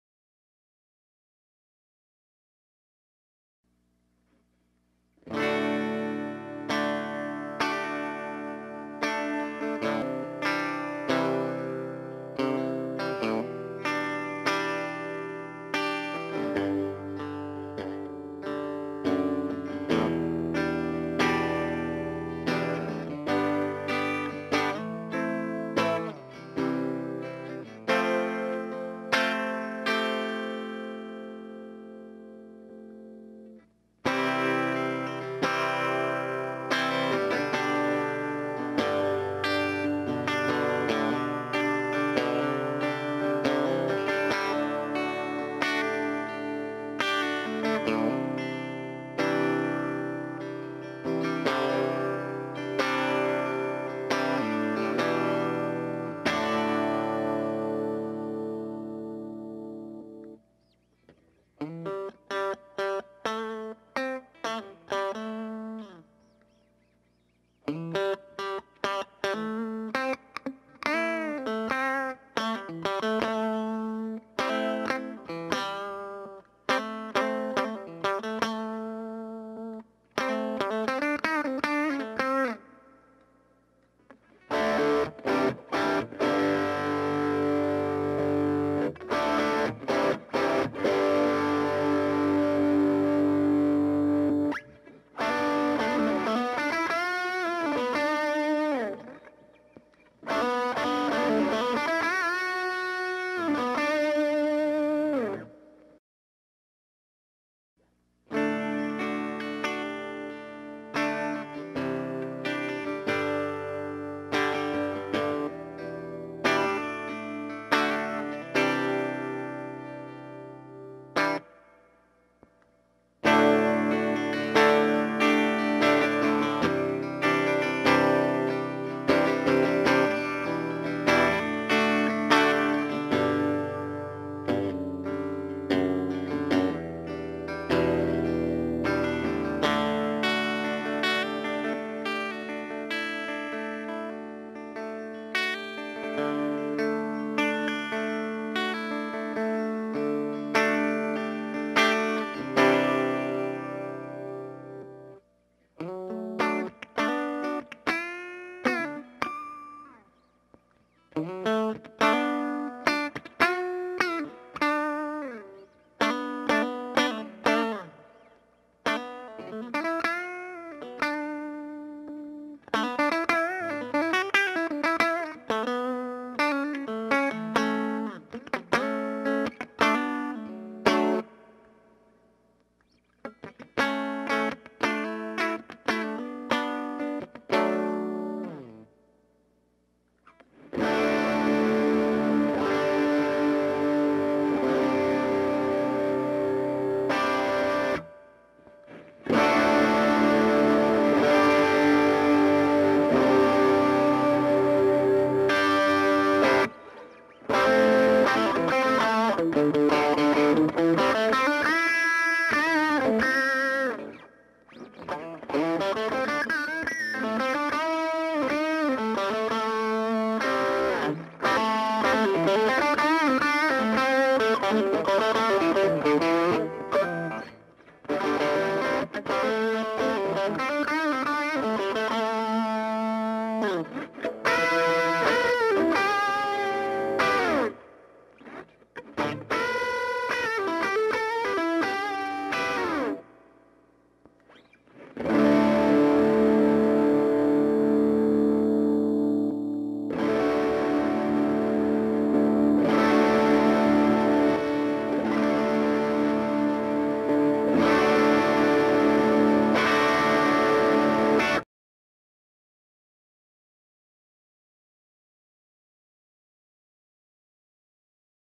Dod fx-84 milkbox compressor demo
Posted for NotImune:

This a comparrison demo of a compressor used first in your chain vs. near the end of your chain. The first half of the video shows the compressor as the first effect, the second half shows the compressor near the end of my chain (guitar, wah, overdrive, distortion, amp - Effects loop, volume pedal, compressor, chorus, tremolo, delay, tube preamp)
Personally i prefer the tone with the compressor near the end, brings more articulation and dynamics to my tone. The pedal first in the chain squishs the tone too much for me, some people like it though! Also the settings to the compressor were idential in both tests, and the overdrive pedal used was a bad monkey tube overdrive. Guitar was a 03' mexican stratocaster using the bridge pickup (dimarzio area 61)